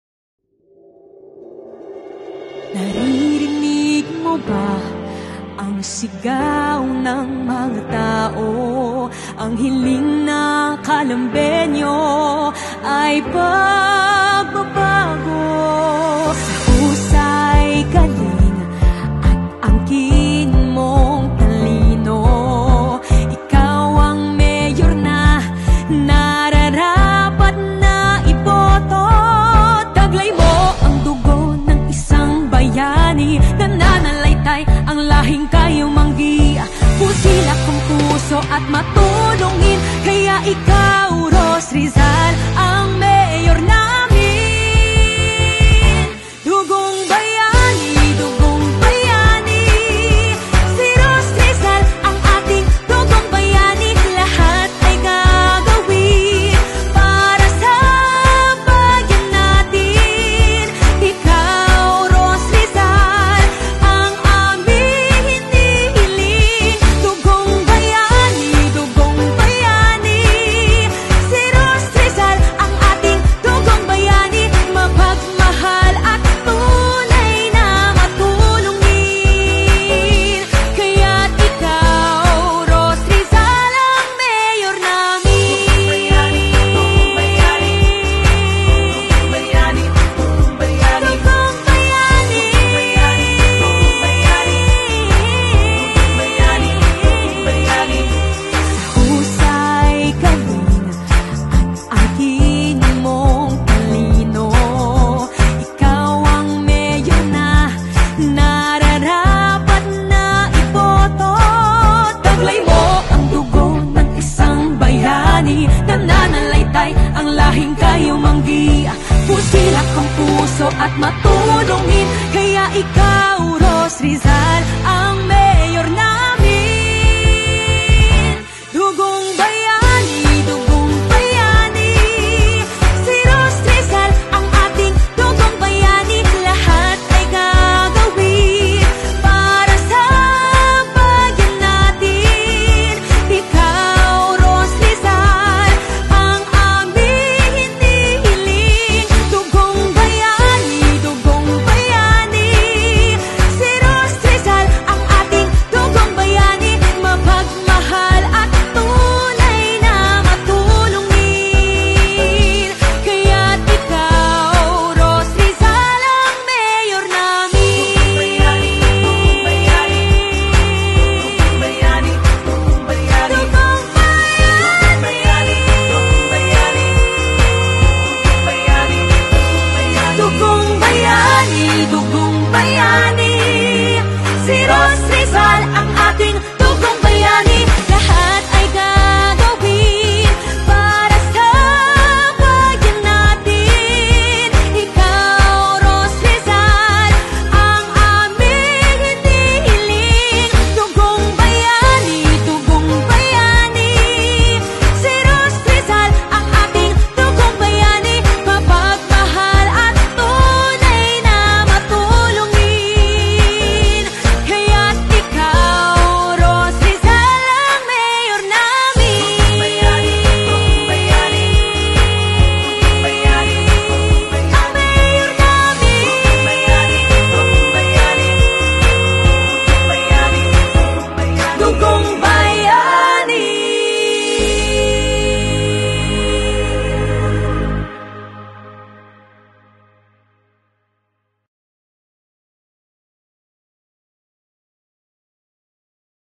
DUGONG BAYANI... IKAW ANG MAYOR NAMIN
Ross Rizal, ang tinaguriang dugong bayani, may hatid na pagbabagong inaasam ng mga Calambeño.